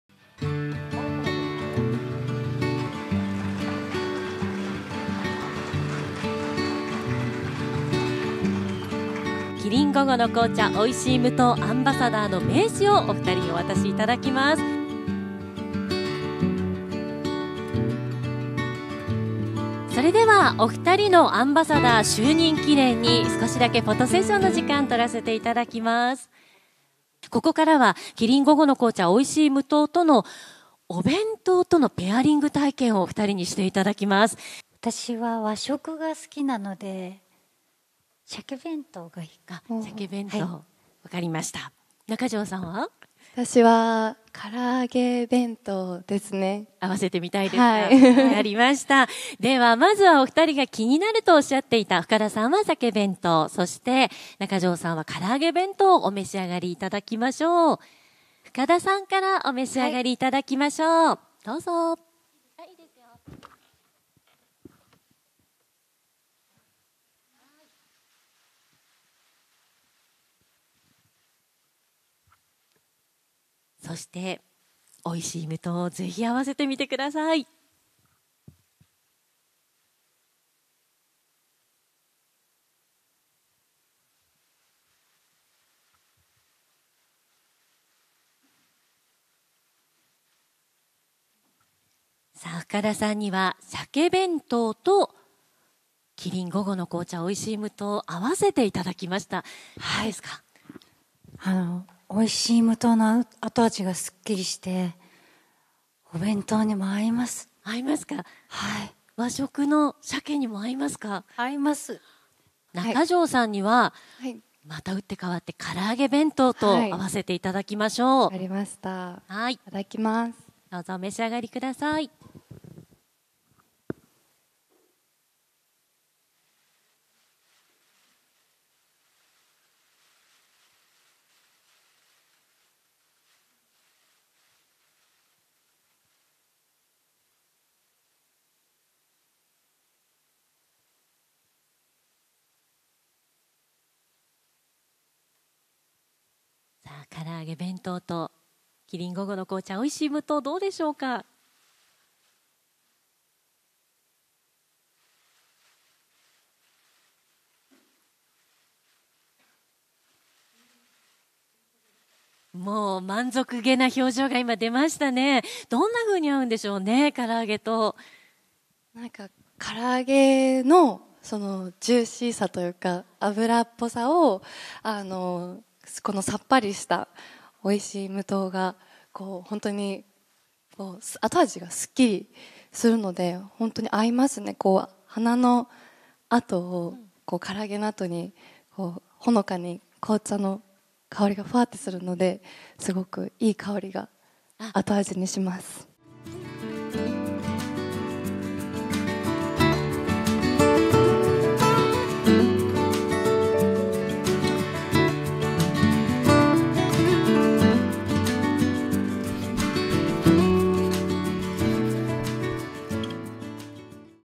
【深田恭子】お弁当と紅茶のペアリング【中条あやみ】
女優深田恭子（39）と中条あやみ（25）が19日、都内で、キリン「午後の紅茶　おいしい無糖」のリニューアル発表会見に出席し、アンバサダーに就任した。2人は20日から放送される新CMに出演する。